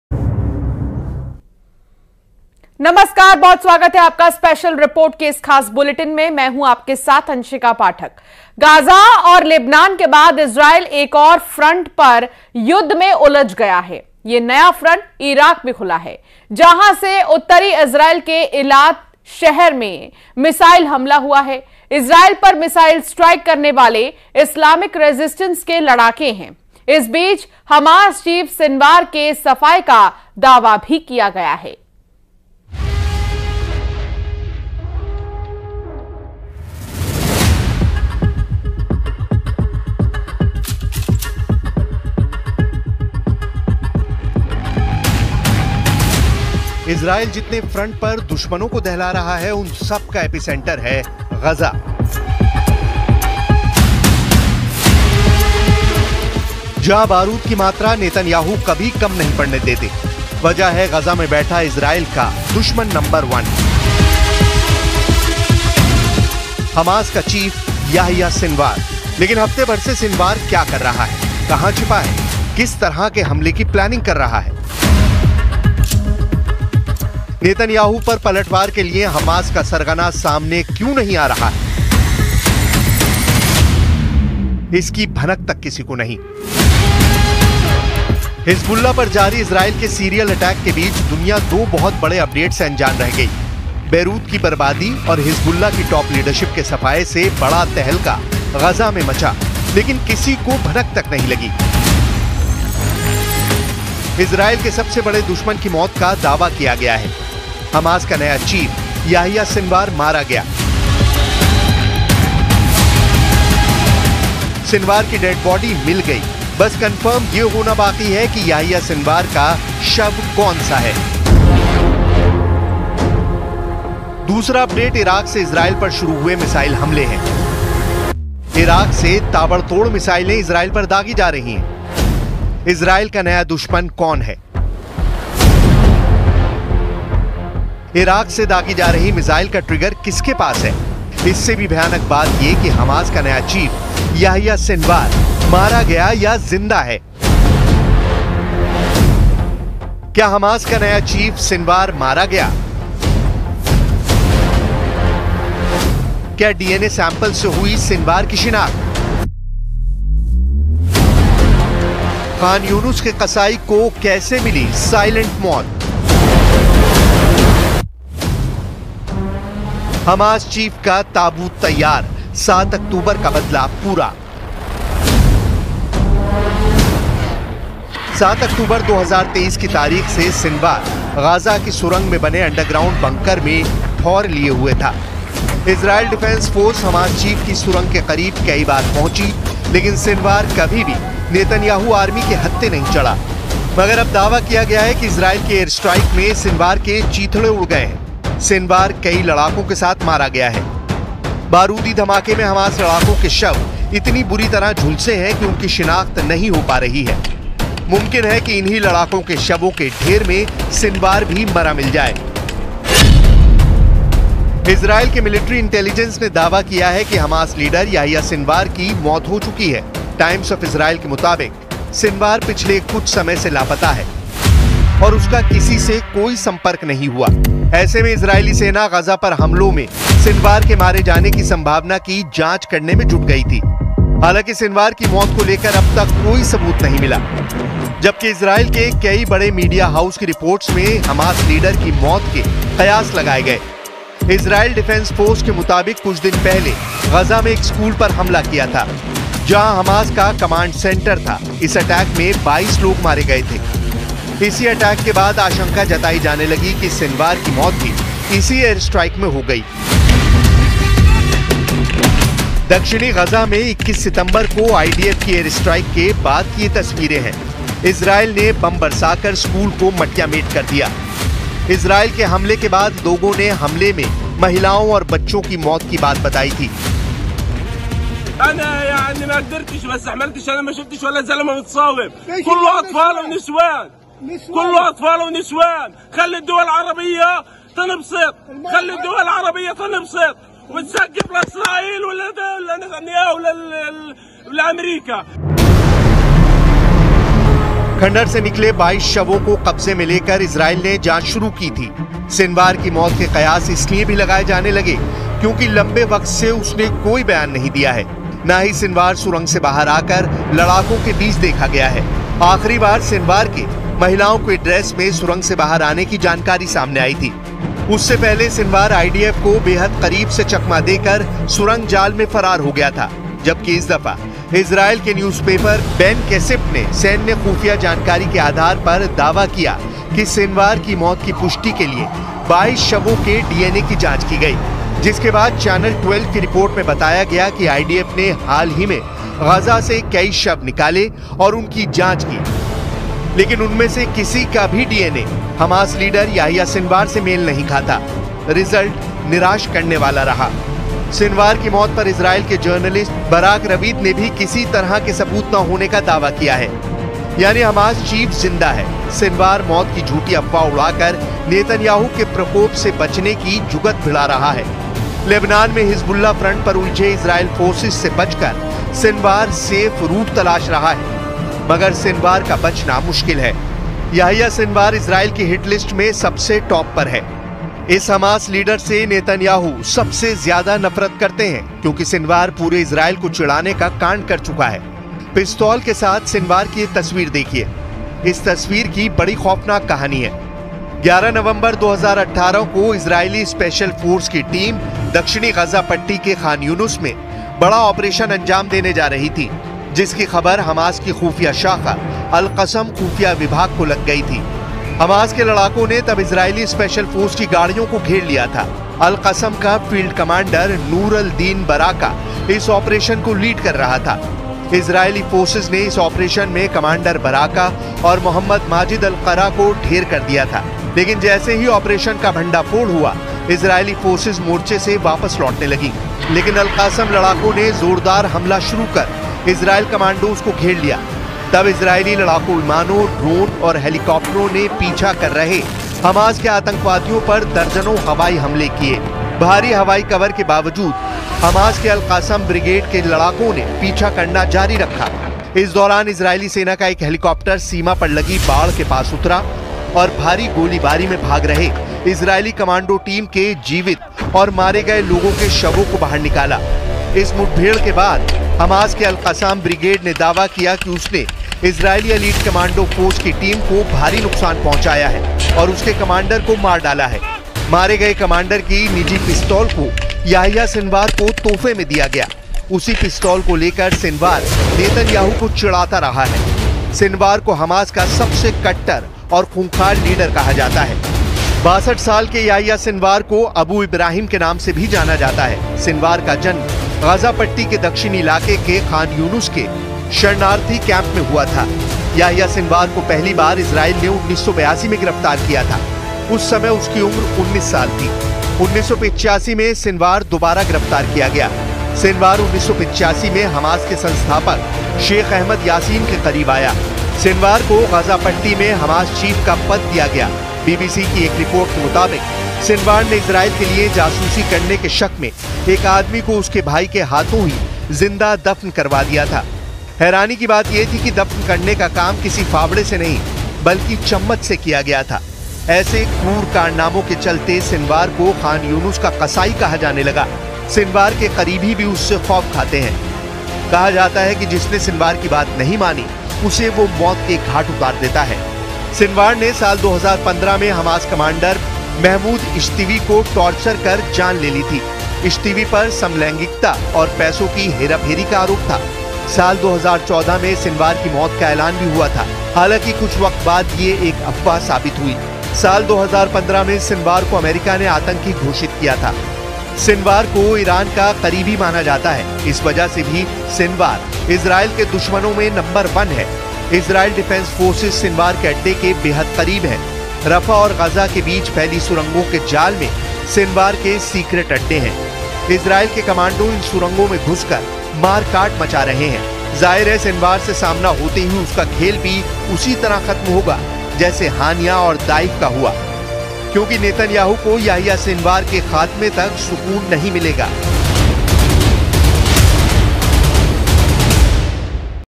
Iran Israel War : Hamas चीफ जिंदा या मुर्दा ? Yahya Sinwar का 'अंडरग्राउंड प्लान'|Tel Aviv | N18G
Israel vs Hezbollah War Update : Gaza और Lebanon के बाद Israel एक और Front पर युद्ध में उलझ गया है. ये नया Front Iraq में खुला है. जहां से उत्तरी इजरायल के Eilat शहर में मिसाइल हमला हुआ है. Israel पर Missile Attack करने वाले Islamic Resistance के लड़ाके हैं. रिपोर्ट देखिए